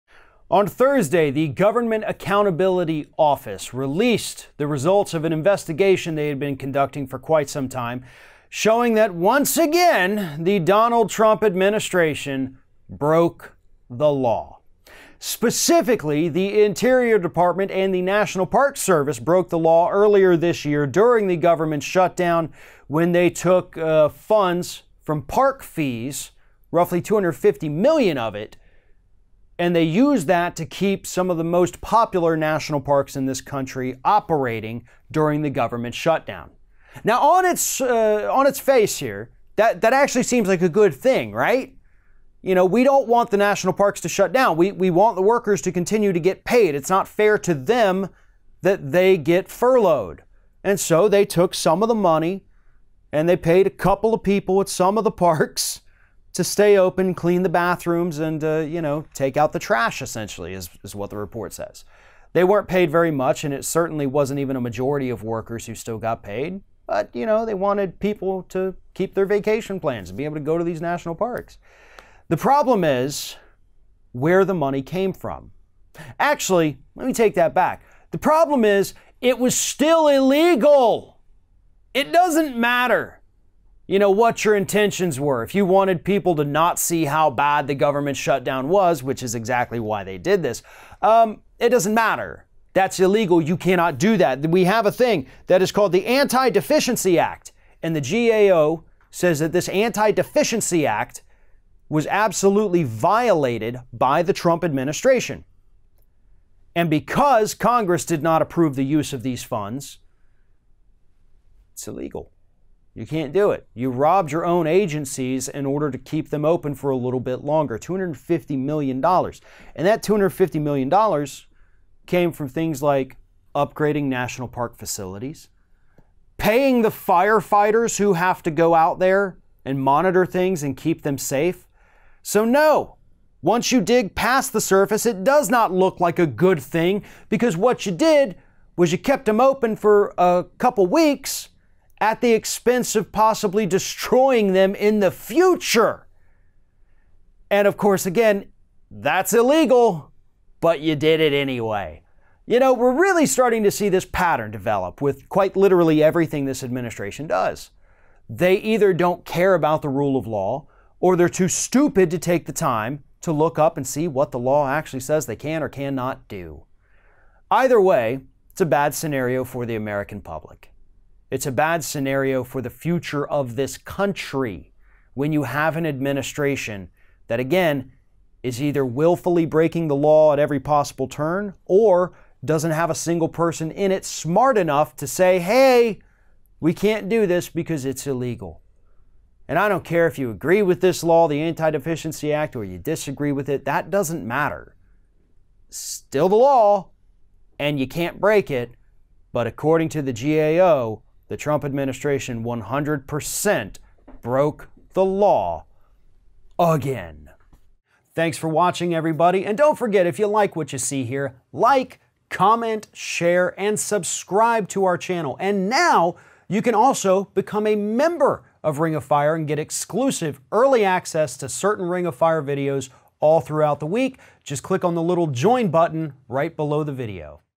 Federal Watchdog Says Trump Admin Broke The Law…Again
The Government Accountability Office released a report on Thursday claiming that the Trump administration broke the law yet again. This time it was the Interior Department and the National Parks Service who used entrance fees that had been set aside to keep the parks running during the government shutdown earlier this year. That may seem like a good thing, but Ring of Fire’s Farron Cousins explains why it is illegal.

On Thursday, the Government Accountability Office released the results of an investigation they had been conducting for quite some time, showing that once again, the Donald Trump administration broke the law, specifically the Interior Department and the National Park Service broke the law earlier this year during the government shutdown when they took a funds from park fees, roughly 250 million of it. And they use that to keep some of the most popular national parks in this country operating during the government shutdown now on it's a on its face here that that actually seems like a good thing, right? You know, we don't want the national parks to shut down. We want the workers to continue to get paid. It's not fair to them that they get furloughed. And so they took some of the money and they paid a couple of people at some of the parks to stay open, clean the bathrooms and a, you know, take out the trash essentially is what the report says.

They weren't paid very much and it certainly wasn't even a majority of workers who still got paid. You know, they wanted people to keep their vacation plans. And be able to go to these national parks. The problem is where the money came from. Actually, let me take that back. The problem is it was still illegal. It doesn't matter. You know what your intentions were. If you wanted people to not see how bad the government shutdown was, which is exactly why they did this, um, it doesn't matter. That's illegal. You cannot do that. That we have a thing that is called the Anti Deficiency Act and the GAO says that this anti deficiency act was absolutely violated by the Trump administration and because congress did not approve the use of these funds, it's illegal. You can't do it. You robbed your own agencies in order to keep them open for a little bit longer, $250 million and that $250 million came from things like upgrading national park facilities, paying the firefighters who have to go out there and monitor things and keep them safe.

So no, once you dig past the surface, it does not look like a good thing because what you did was you kept them open for a couple of weeks at the expense of possibly destroying them in the future. And of course, again, that's illegal, but you did it anyway. You know, we're really starting to see this pattern develop with quite literally everything this administration does. They either don't care about the rule of law or they're too stupid to take the time to look up and see what the law actually says they can or cannot do. Either way, it's a bad scenario for the American public. It's a bad scenario for the future of this country. When you have an administration that again is either willfully breaking the law at every possible turn or doesn't have a single person in it, smart enough to say, hey, we can't do this because it's illegal and I don't care if you agree with this law, the Anti Deficiency Act or if you disagree with it, that doesn't matter. Still the law and you can't break it, but according to the GAO, the Trump administration 100% broke the law again.